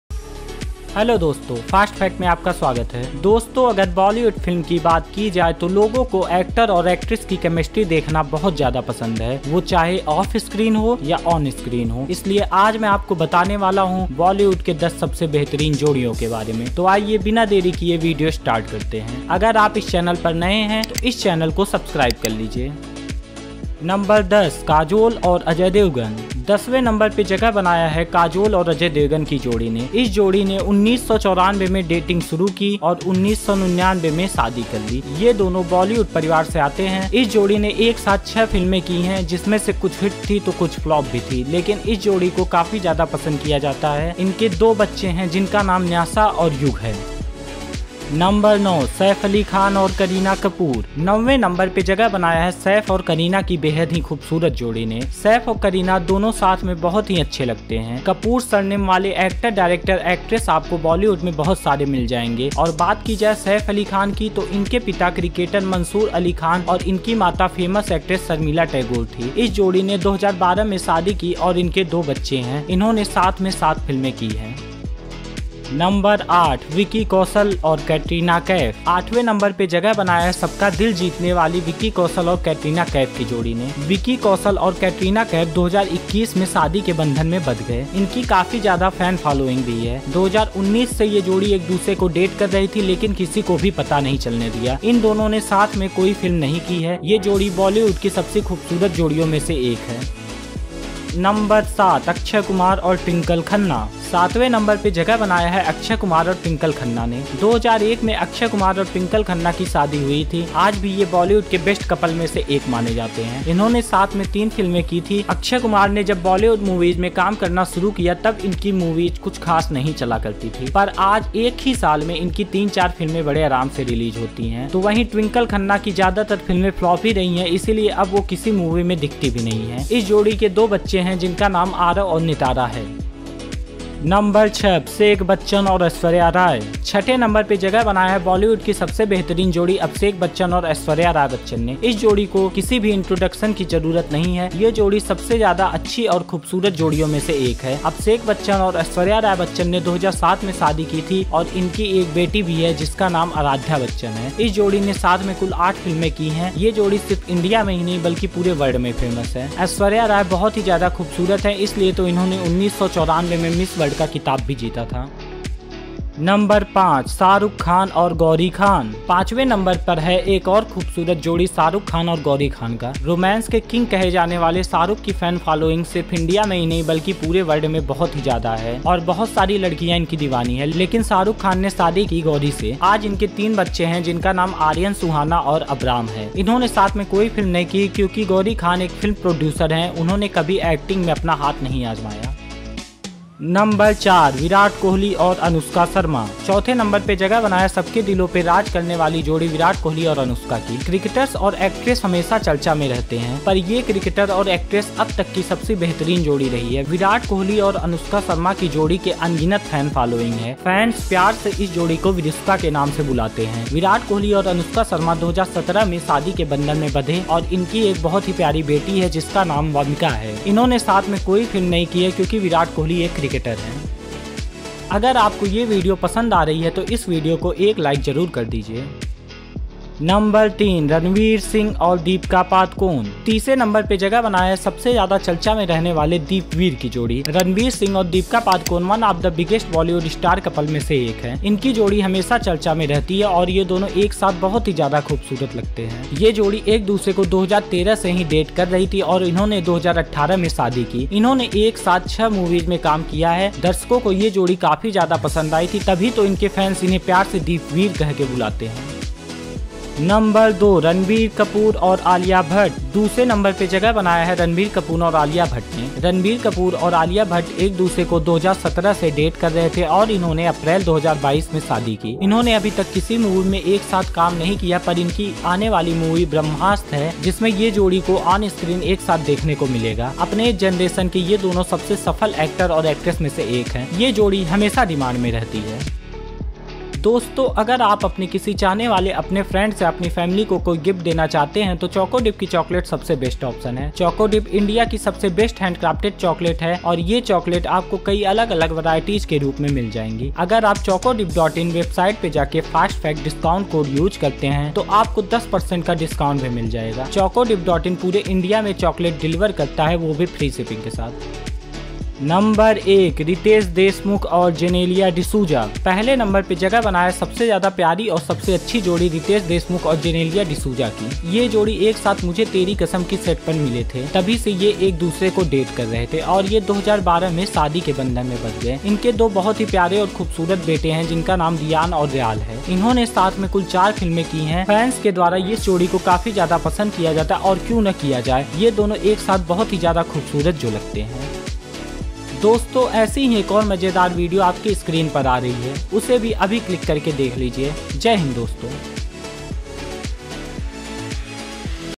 Most Beautiful Couples Of Bollywood | बॉलीवुड की सबसे अच्छी जोड़ी | Fast Facts
Let us find out 10 Best Couples Of Bollywood!

Hello friends, today in this video I am going to tell you about the best Bollywood couples. Who is the best couple in Bollywood? Do Deepika Padukone and Ranveer Singh look great together? Who is the cutest couple of bollywood? Which Bollywood couple is the best, be it on screen or off screen? Will Salman Khan ever get married? Should cricketers and actresses marry each other? Which was the most expensive Bollywood wedding ever? Are Virat Kohli and Anushka Sharma the best pairing? Abhishek Bachchan and Aishwarya Rai Bachchan both look so cute together? Which Bollywood couple are the most romantic couples in real life too? In this video you will get all the information about the Bollywood couples and when they got married and how many children they have.

See you in the next amazing video filled with knowledge and entertainment